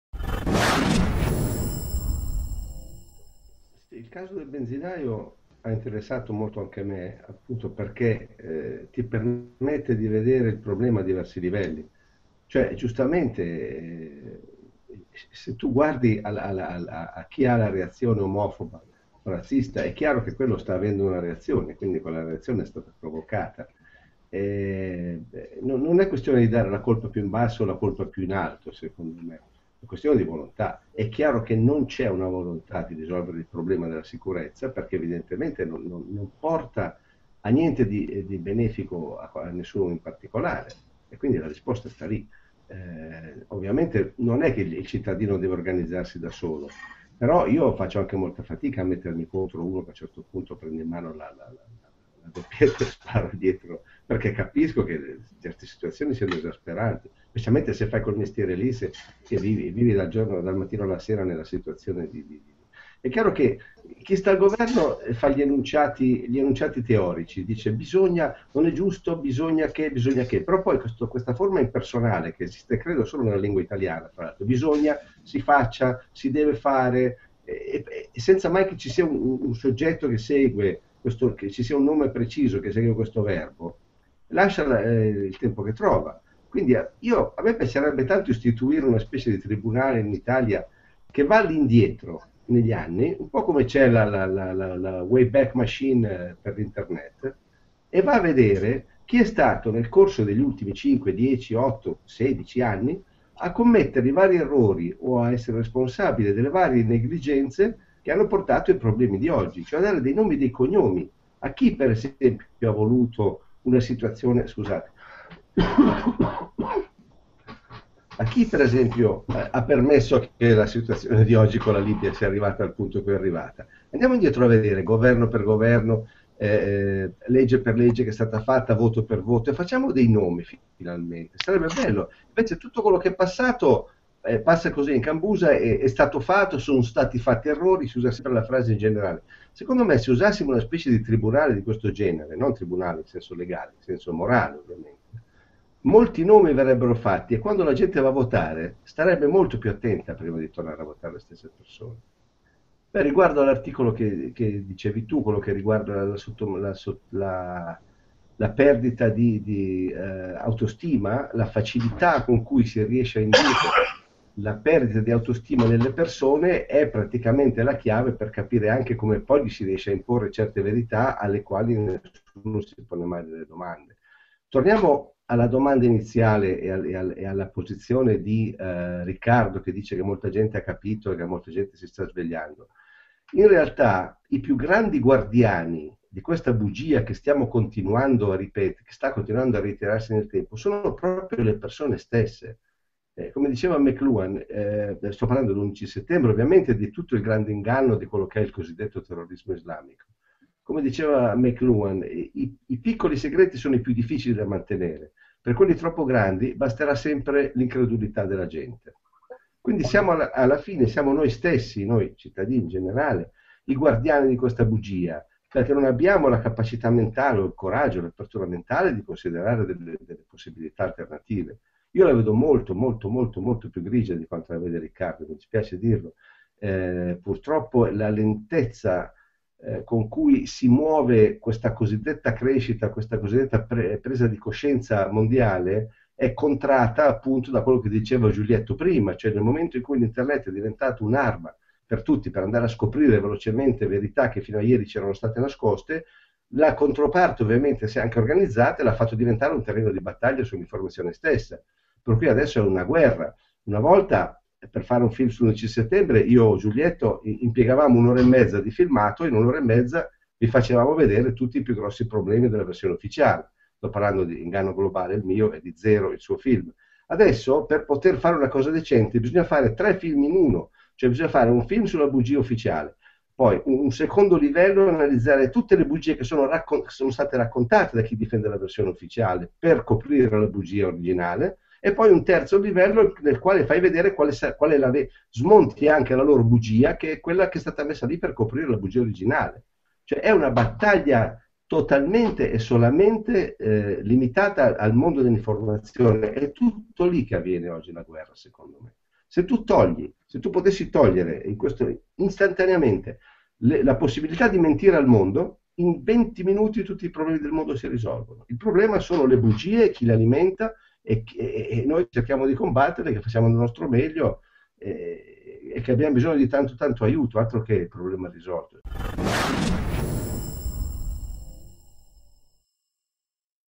HRN Live - Massimo Mazzucco -omofobia sicurezza e informazione e politica-
Il regista Massimo Mazzucco , interagisce in video conferenza in diretta con la redazione di HrnLive ,durante la puntata del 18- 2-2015 ),in proposto agli scenari internazionali che si vanno delineando .questo passaggio in particolare rispondendo alle domande della chat del programma spiega omofobia sicurezza e informazione

Il portale del libero pensiero!#
Musica, Cultura, Idee, Notizie